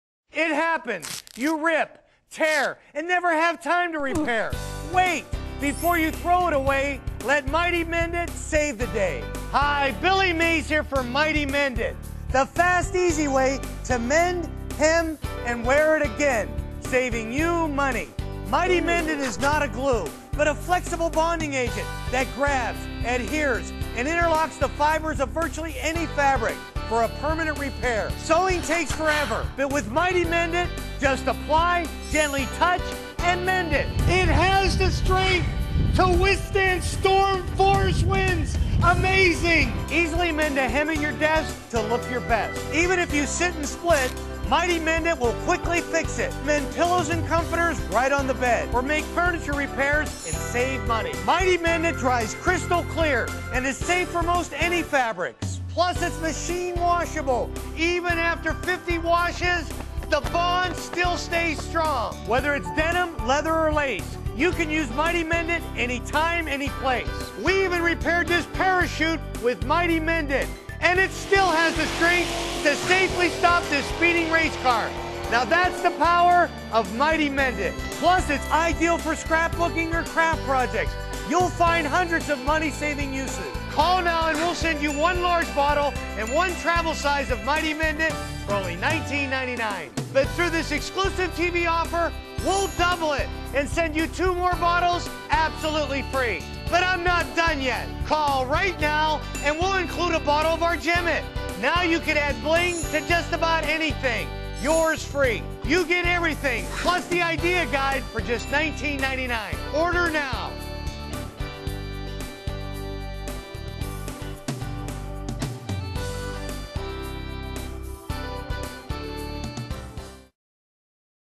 Mighty Mendit Commercial 120 v18
Mighty Mendit commercial featuring Billy Mays produced for Top Dog Direct by Huttton Miller